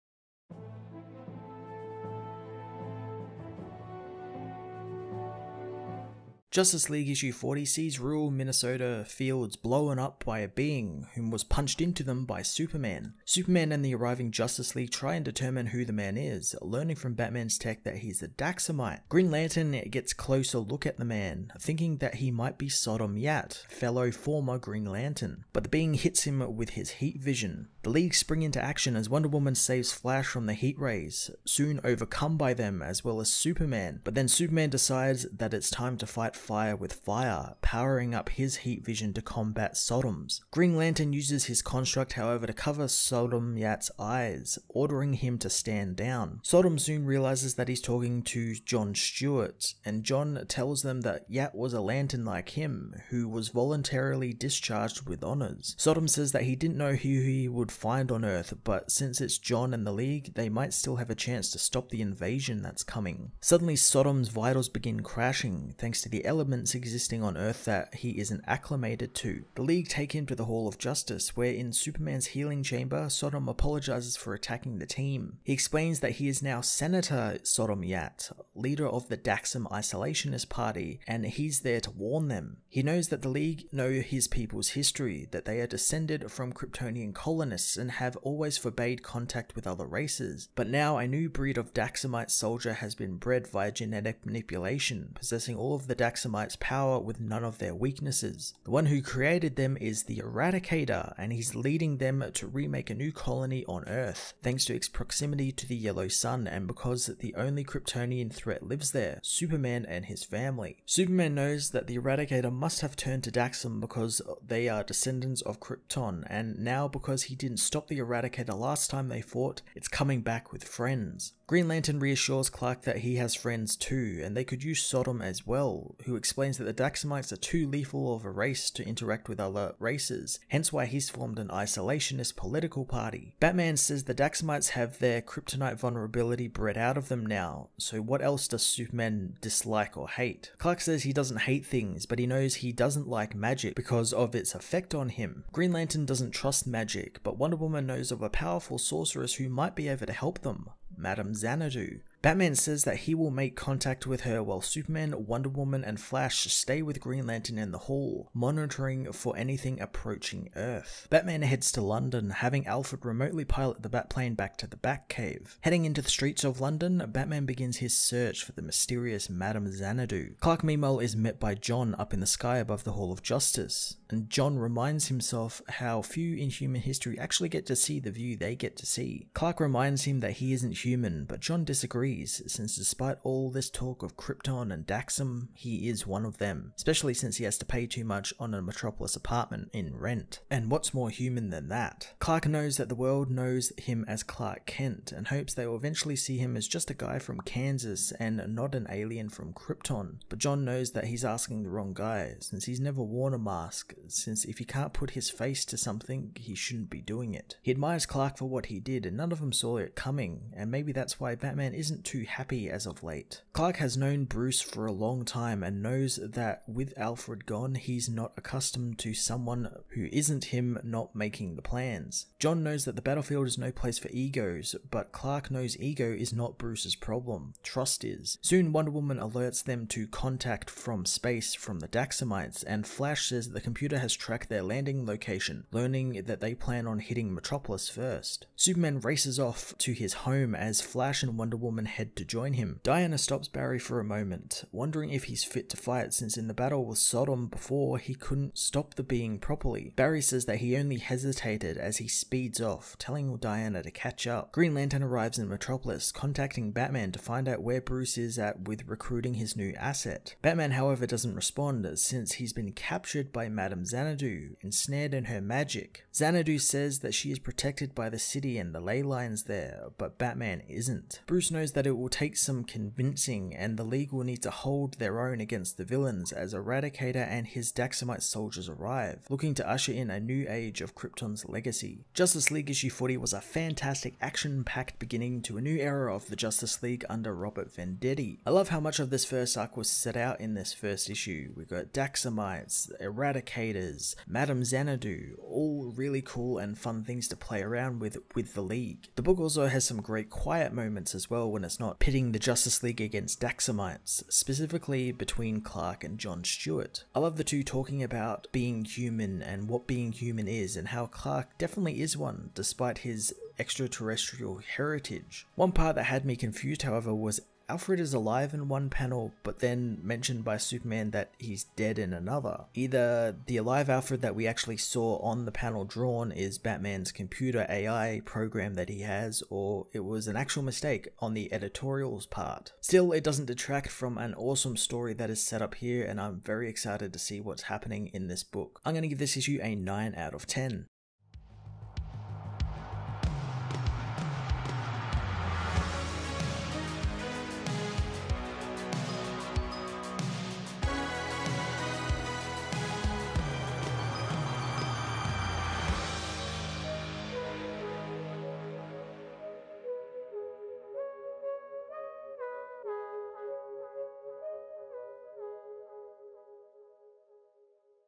Invasion of The Supermen | Justice League #40 Review (Robert Venditti First Issue)
The Justice League learn of an impending invasion of Earth from the former Green Lantern Sodom Yat, but can the heroes gather enough power to match the might of The Eradicator and his army of Daxamite super soldiers?